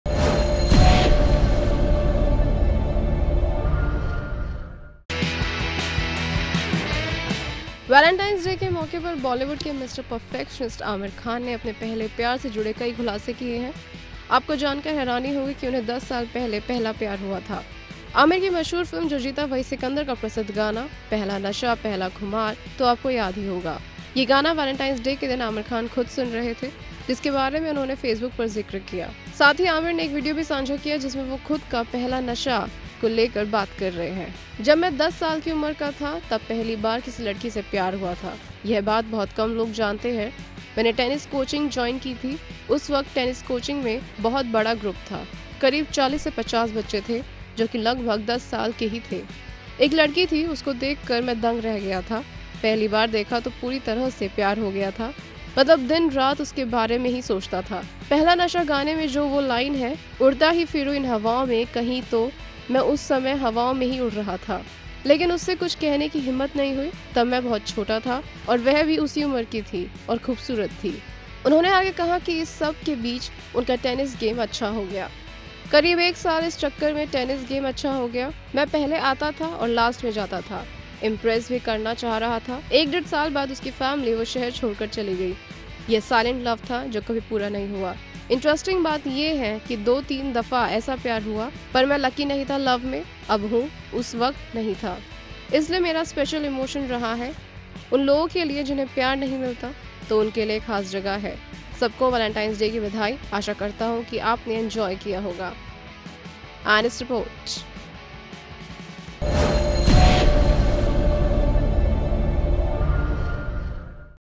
10 साल की उम्र में Aamir Khan को हुआ था "पहला प्यार" , इस वैलेंटाइन डे किया खुलासा
वैलेंटाइन डे के मौके पर बॉलीवुड के मिस्टर परफेक्शनिस्ट आमिर खान ने अपने पहले प्यार से जुडे कई खुलासे किये हैं।  आपको जानकर हैरानी होगी कि उन्हें 10 साल पहले पहला प्यार हुआ था।